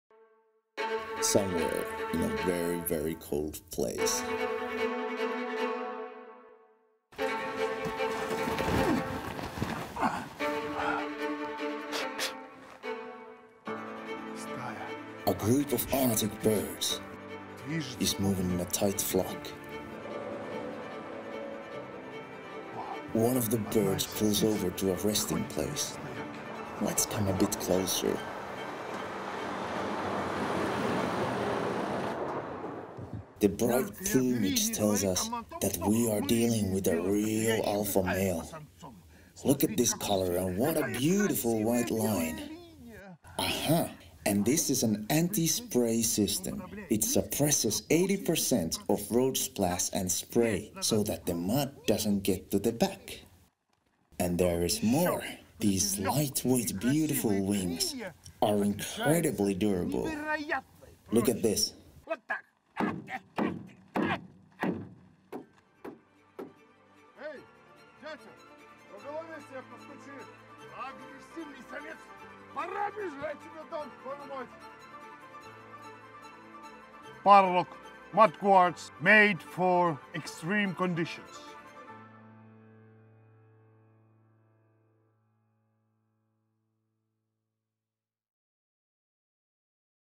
Parlok Mudguards - The Alpha Male (dubbed with subtitles eng + fin)
🇫🇮 Parlok Mudguards - The Alpha Male (dubbed). Starring Ville Haapasalo. Read more about our company:

Parlok. Finnish mudguards made for extreme conditions. 🇫🇮